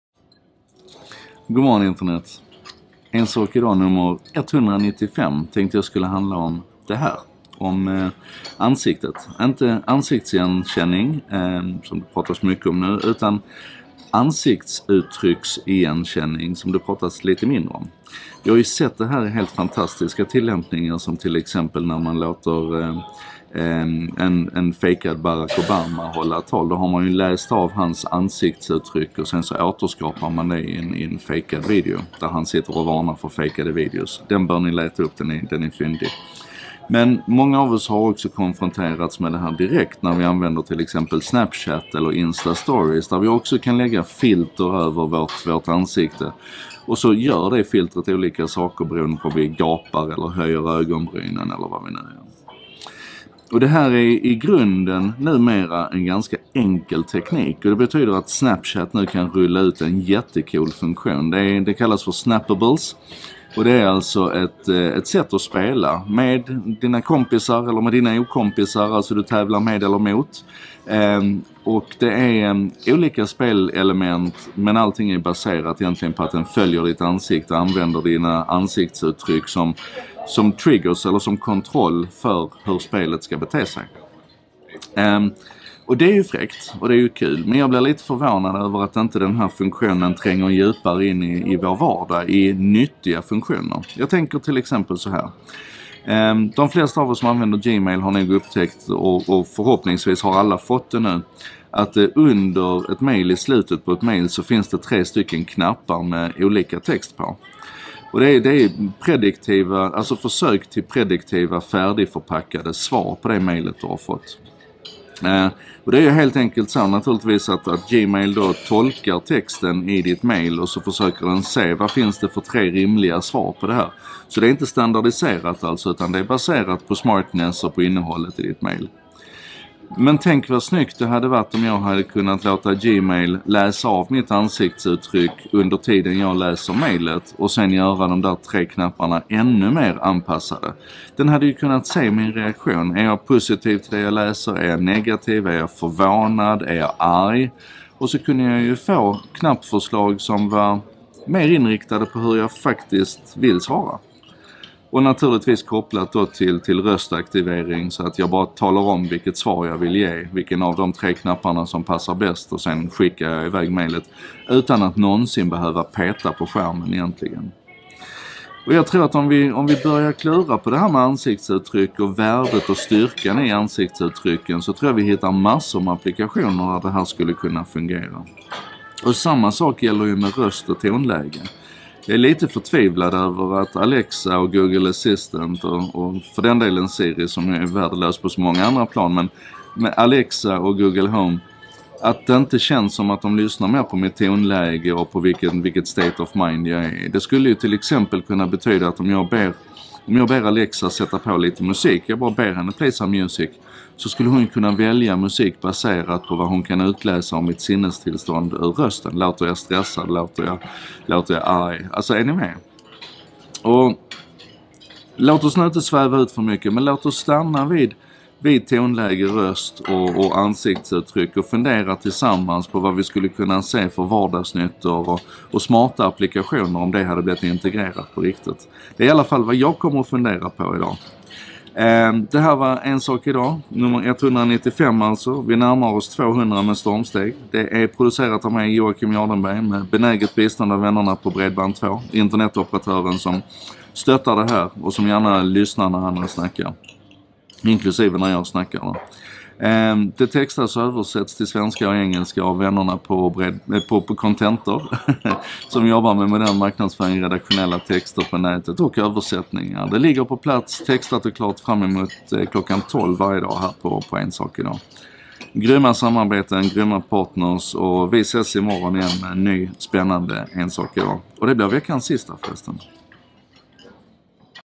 195. Dina ansiktsuttryck - din nästa fjärrkontroll?
Om Snapchats nya spelfunktion som styrs av ditt ansikte. Om hur ansiktsuttryck, tillsammans med lite smartness från maskinerna kan användas för att styra spel och fejka videos. Men också om hur det borde kunna komma till rejäl nytta, redan nu. Ett ansiktsuttryck säger mer än tusen ord?
Det här inslaget har skapats av Joakim Jardenberg med stöd från Bredband2 och Contentor. Tusen tack till dem, och till alla er som kommenterar och delar - och aktivt deltar i vårt gemensamma lärande.